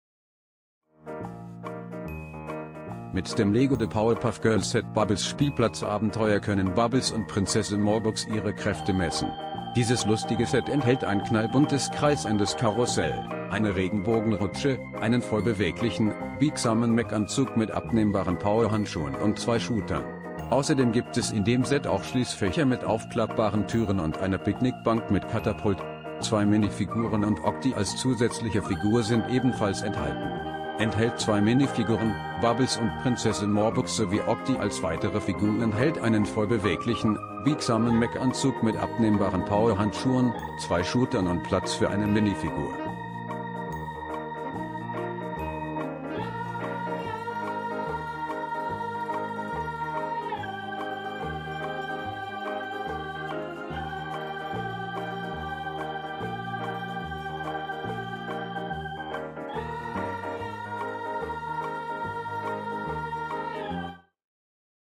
Lego Bubbles' Spielplatzabenteuer 41287: Rezension
Mit dem LEGO The Powerpuff Girls Set „Bubbles‘ Spielplatzabenteuer“ können Bubbles und Prinzessin Morbucks ihre Kräfte messen! Dieses lustige Set enthält ein knallbuntes kreisendes Karussell, eine Regenbogenrutsche, einen voll beweglichen, biegsamen Mech-Anzug mit abnehmbaren Power-Handschuhen und 2 Shootern. Außerdem gibt es in dem Set auch Schließfächer mit aufklappbaren Türen und eine Picknickbank mit Katapult. 2 Minifiguren und Octi als zusätzliche Figur sind ebenfalls enthalten. Enthält 2 Minifiguren: Bubbles und Prinzessin Morbucks sowie Octi als weitere Figur Enthält einen voll beweglichen, biegsamen Mech-Anzug mit abnehmbaren Power-Handschuhen, 2 Shootern und Platz für eine Minifigur.